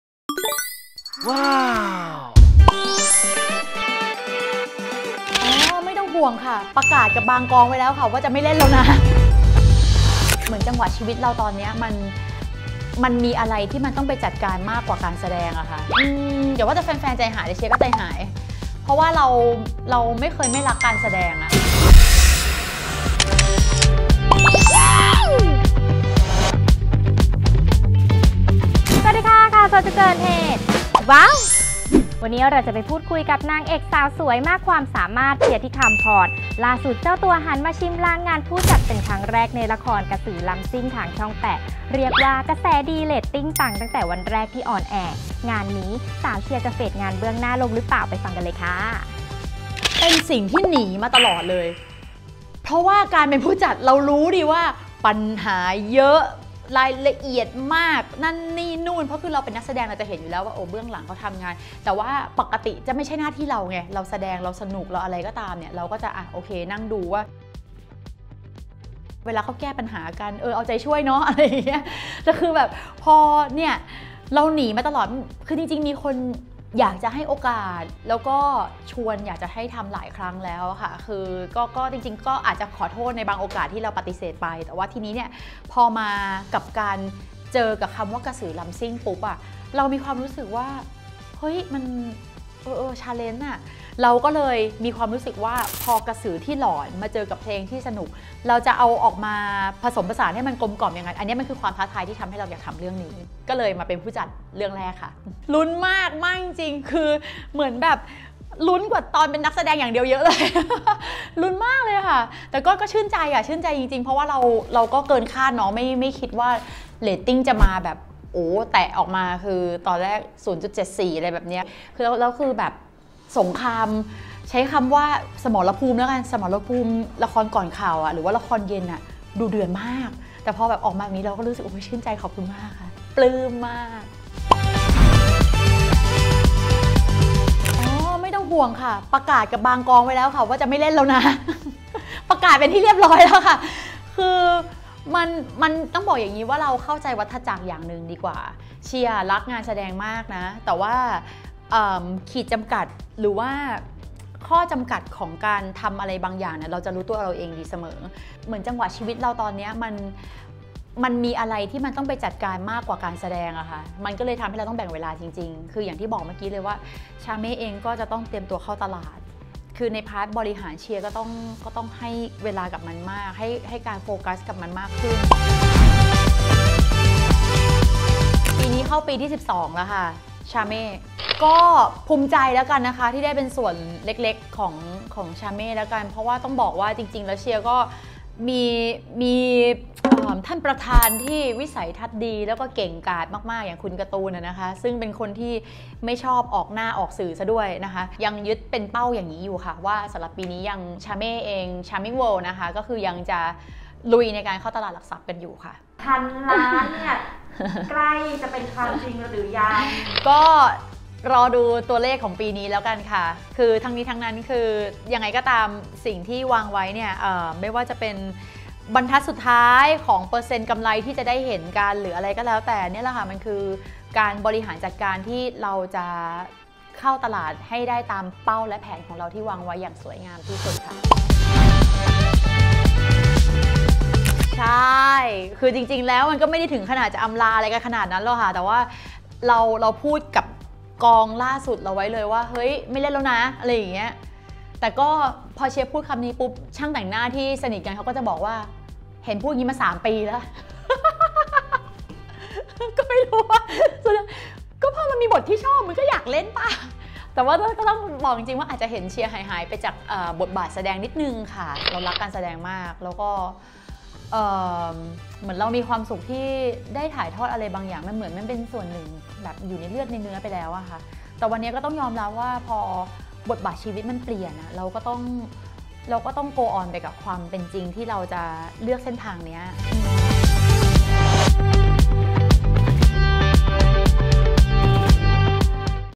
“เชียร์ ฑิฆัมพร” แย้มเตรียมลาวงการ หันไปลุยธุรกิจพันล้าน : Khaosod TV
“เชียร์ ฑิฆัมพร” แย้มเตรียมลาวงการ หันไปลุยธุรกิจพันล้าน
สนับสนุนคลิปโดย ฟาร์มเฮ้าส์ สั่งขนมปัง คุกกี้ เบเกอรี่กว่า 100 ชนิดง่ายๆ ผ่านไลน์ฟาร์มเฮ้าส์เดลิเวอรี่ : @farmhousethai หรือ คลิก